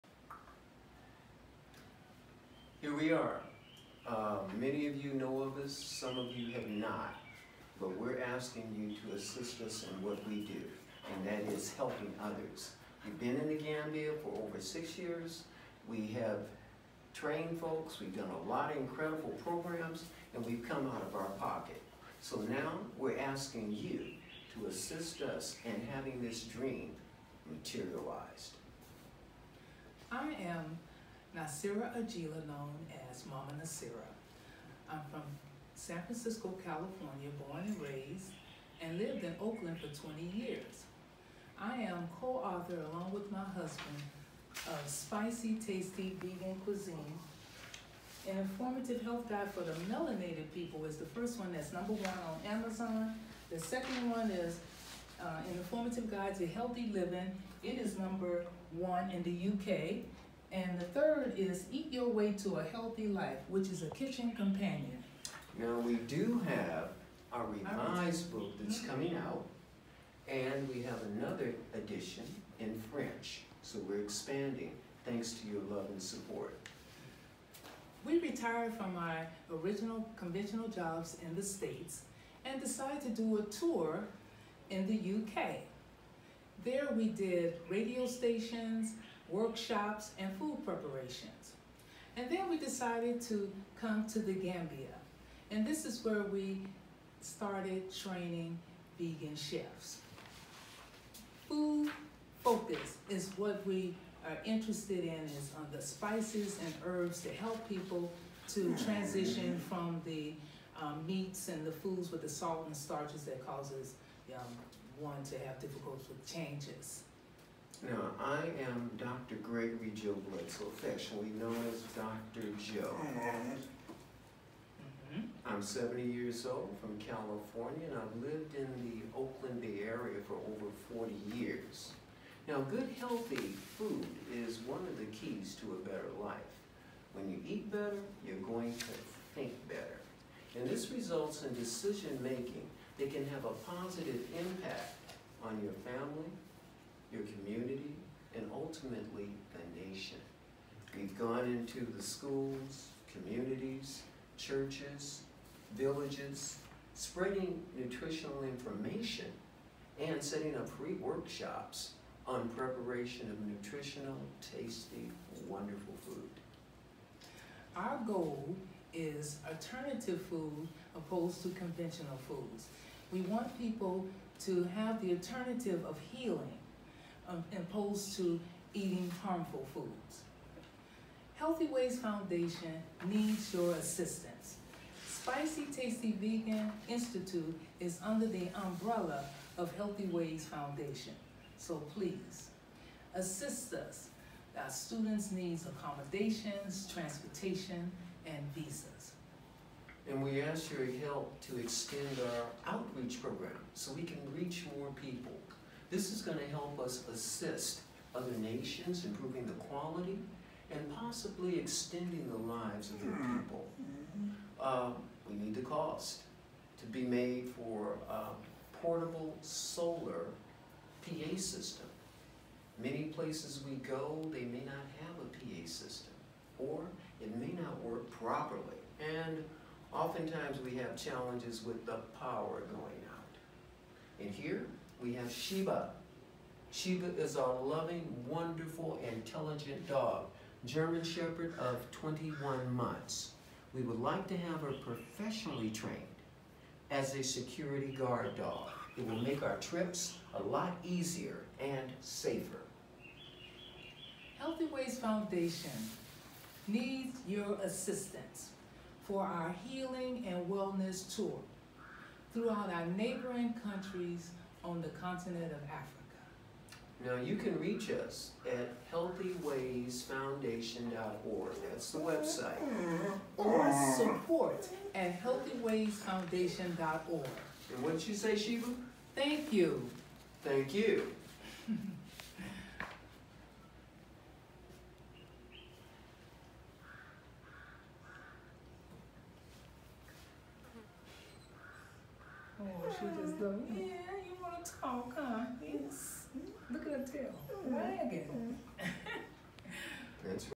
Healing & Wellness Tour Appeal
Mama Naasira and Dr. Joe explain why the Healing and Wellnes Tour needs your support. First 10 donators get free copy of upcoming Revised Edition of the Book.